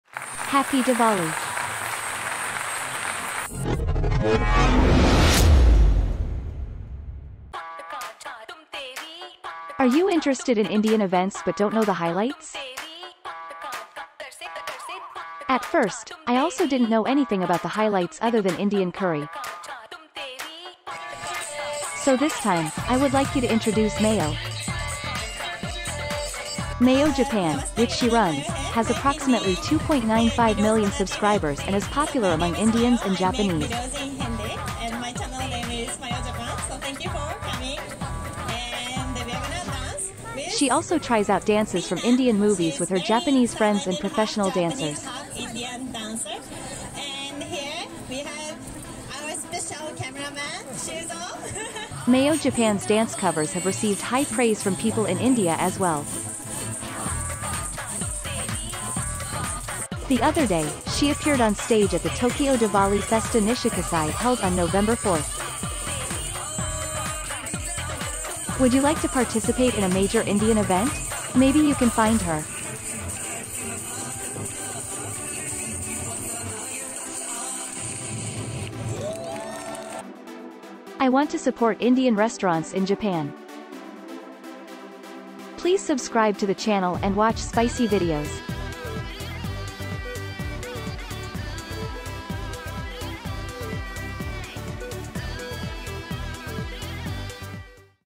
YouTuber Mayo Japan dance is amazing! Tokyo Diwali Festa Nishikasai 2023
Happy Diwali!

Are you interested in Indian events but don't know the highlights?

At first, I also didn't know anything about the highlights other than Indian curry.

So this time, I would like you to introduce Mayo.

Mayo Japan, which she runs, has approximately 2.95 million subscribers and is popular among Indians and Japanese.

She also tries out dances from Indian movies with her Japanese friends and professional dancers.

Mayo Japan's dance covers have received high praise from people in India as well.

The other day, she appeared on stage at the Tokyo Diwali Festa Nishikasai held on November 4th.

Would you like to participate in a major Indian event?  Maybe you can find her.

I want to support Indian restaurants in Japan.

■Event name
Tokyo Diwali Festival at Nishikasai

■Place
Shinden 6 Go Park.
(8-6 Nishikasai, Edogawa-ku, Tokyo)
*5 minutes walk from the south exit of Nishikasai Station on the Tokyo Metro Tozai Line

■How to apply, admission fee
No application required. Admission is free.

■ Organizer
Diwali Festa Nishikasai Executive Committee

Voice by Ondoku3